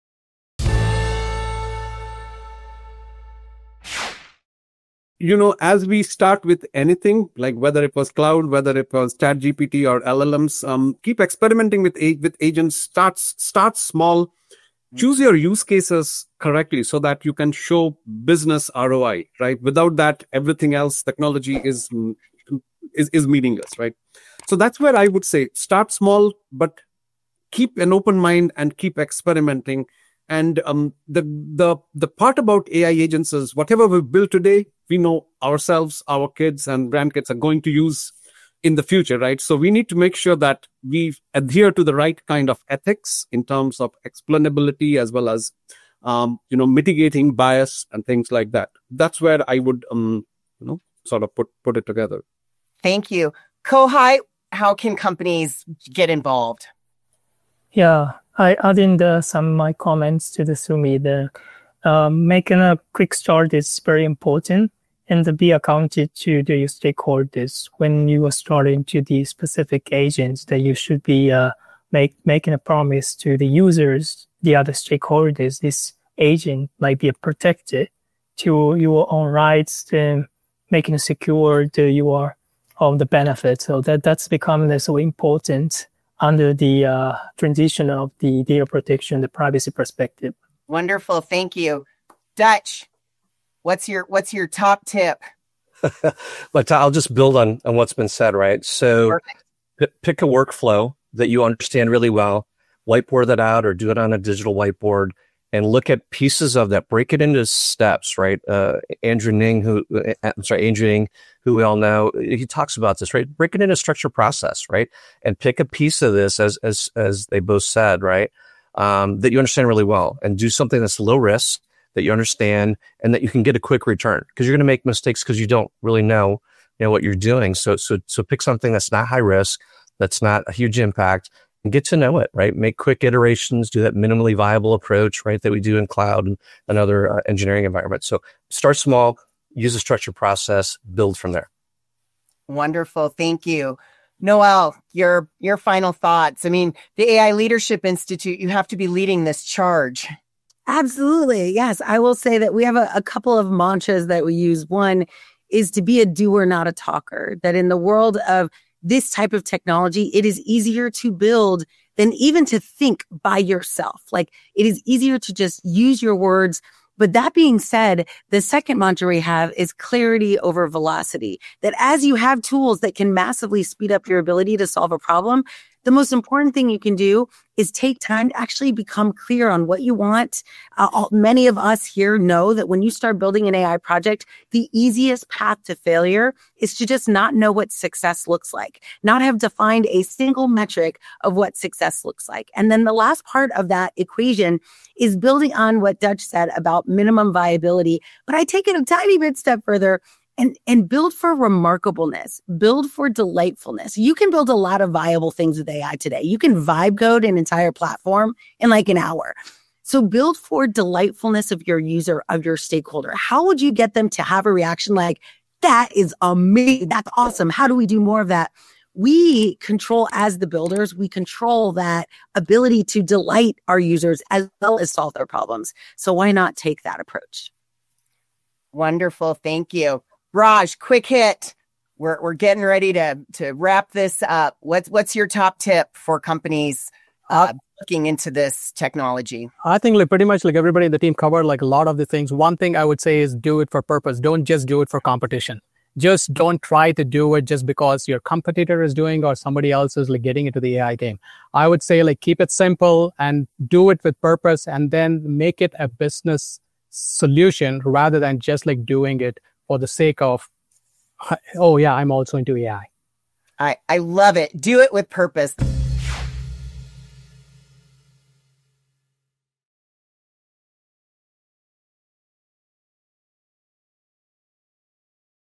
AI Industry Leaders Share Their Top AI Agent Implementation Tips
Join the Insight Jam today!

What are the best use cases to start with when deploying AI agents?
How do you choose low-risk, high-impact AI agent projects?

Top AI experts share their battle-tested strategies for successfully implementing AI agents in business, from choosing the right use cases to building stakeholder trust. They reveal how to start small with agentic AI while maximizing ROI, ensuring ethical implementation, and creating delightful user experiences that drive real business transformation. Perfect for executives ready to move beyond AI experimentation to strategic deployment.

This is a key takeaway from our Q2 2025 Mini-Jam panel: AI Agents in Proactive Data Management: Governance, Ethics, Architecture.

• PANELISTS •
Cher Fox - Founder, Fox Consulting
Dutch Schwartz - VP of Cloud Services, SideChannel
Kohei Kurihara - CEO, Privacy By Design Lab
Noelle Russell - CEO, AI Leadership Institute
Sumit Pal - Strategic Technology Director, Graphwise

• CHAPTERS •
0:00 Start Small with Proven ROI and Ethical AI Implementation
0:54 Accountability and Data Protection in AI Agent Deployment
1:30 Workflow Mapping and Low-Risk AI Agent Development
2:30 Clarity Over Velocity and Building for User Delight
4:02 Purpose-Driven AI Beyond Just Competition